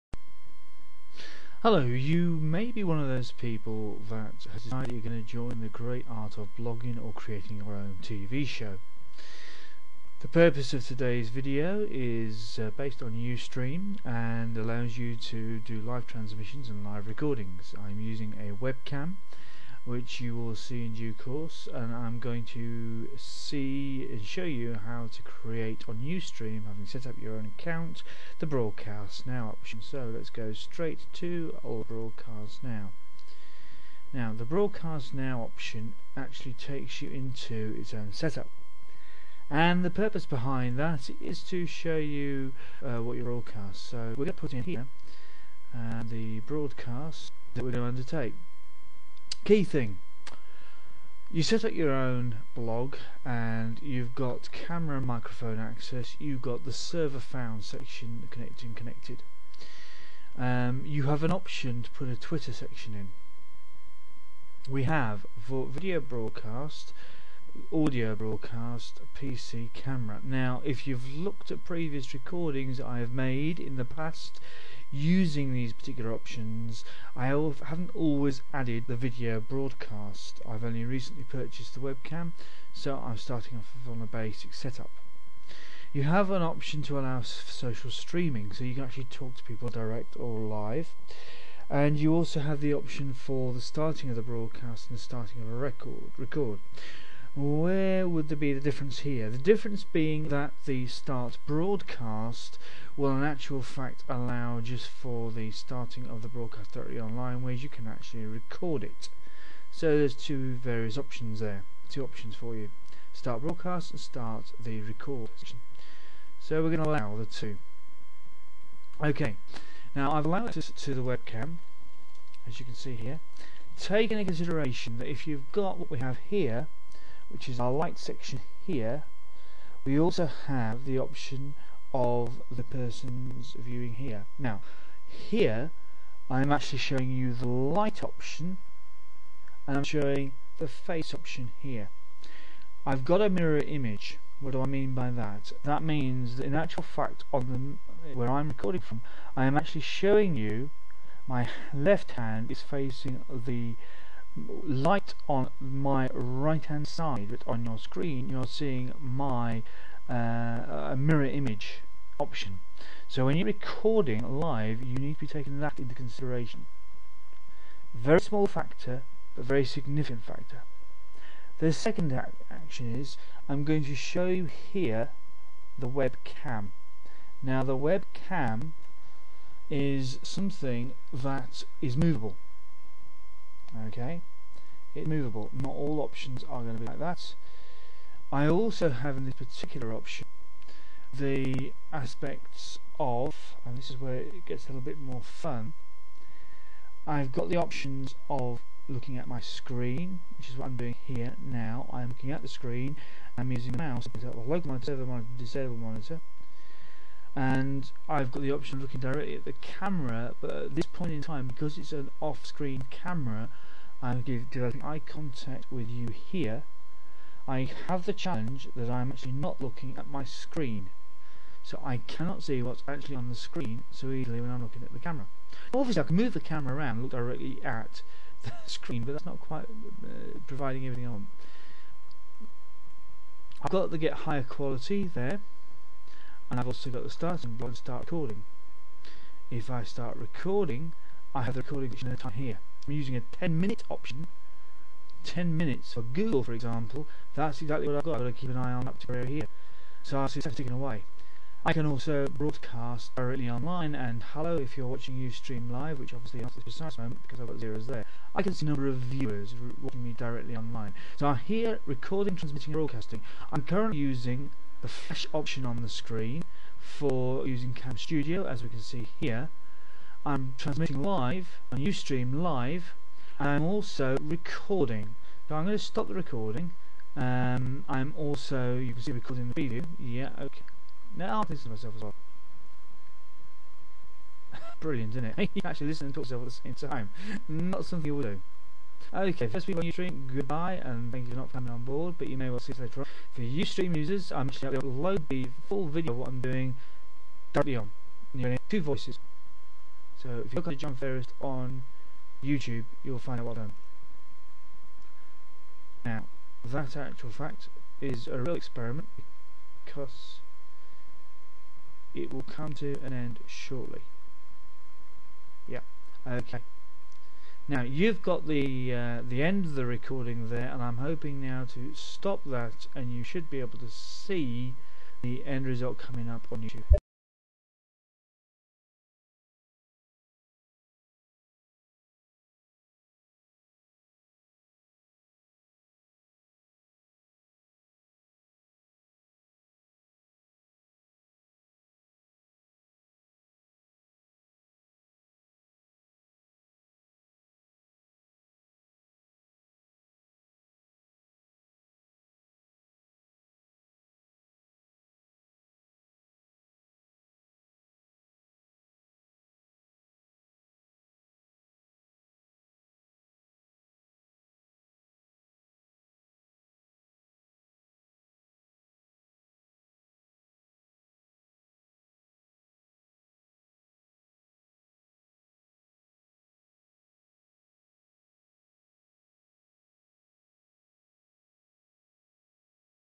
setup a web cam to record on Ustream the recording could be over 10mins
let's watch what happens on screen as we set up a recording direct to Ustream. I will show you some of the challenges of recording off a 2pound web cam and the different web settings that are accesible